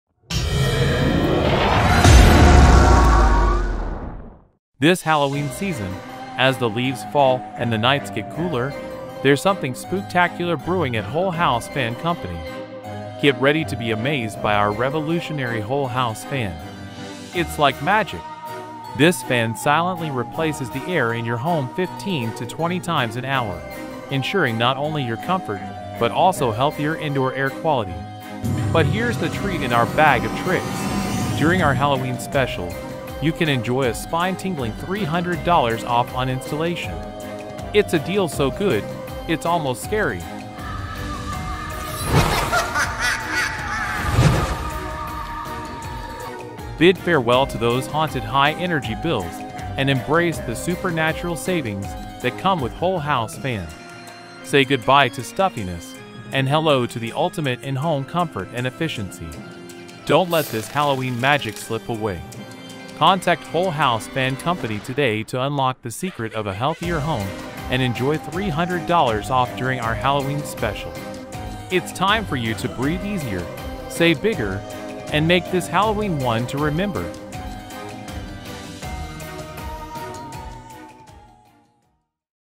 Halloween Special: Get $300 Off Whole House Fan Installation and Breathe Easy
This Halloween, treat yourself to ultimate home comfort and supernatural savings! Discover the magic of Whole House Fan Installation - silently refreshing your home's air up to 20 times an hour. And here's the spellbinding part: during our Halloween Special, you can enjoy a ghostly $300 off on installation. Say goodbye to high energy bills and stuffy indoor air.